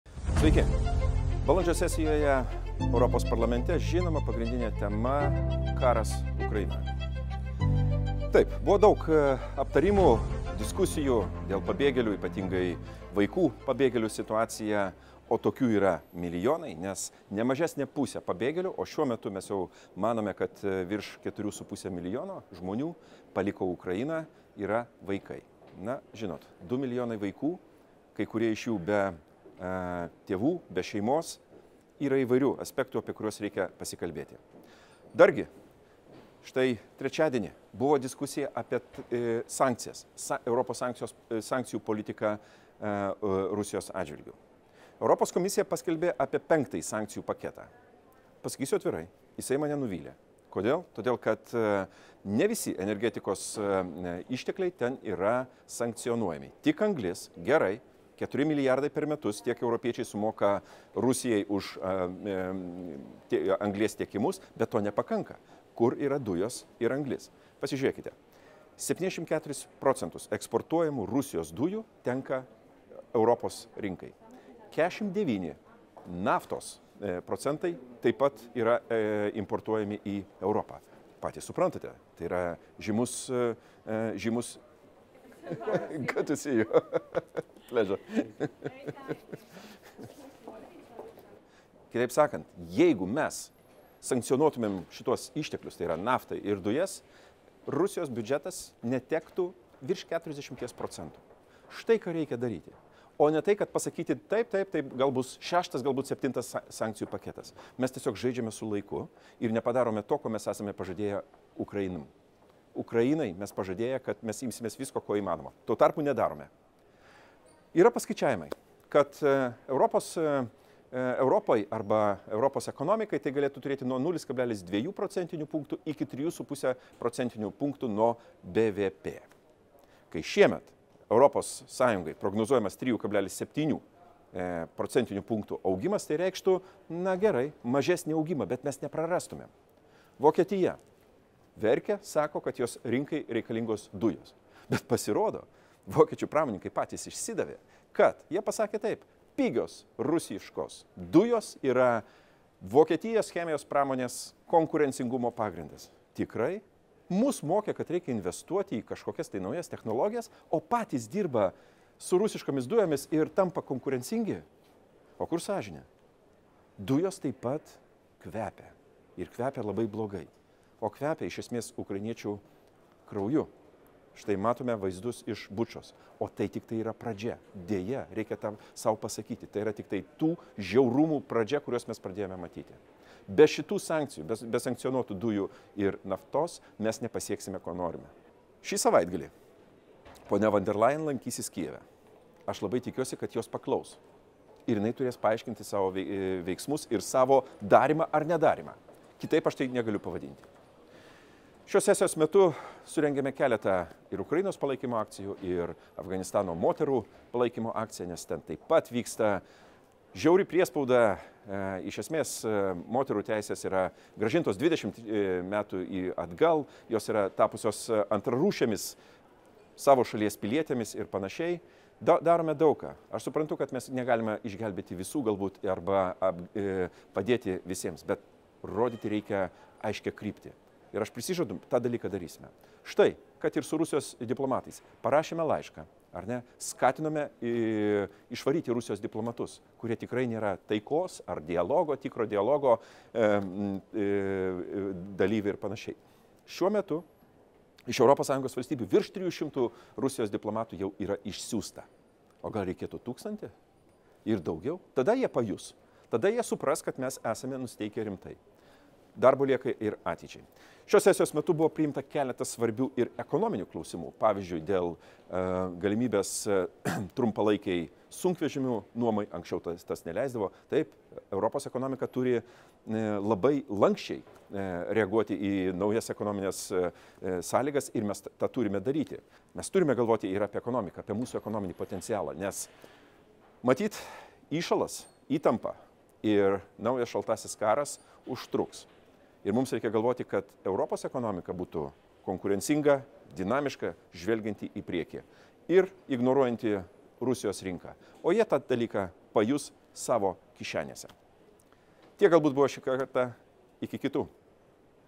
Strasbūrodienoraštis 2022.04.08
apie svarbiausius šios savaitės politinius įvykius Europos Parlamente.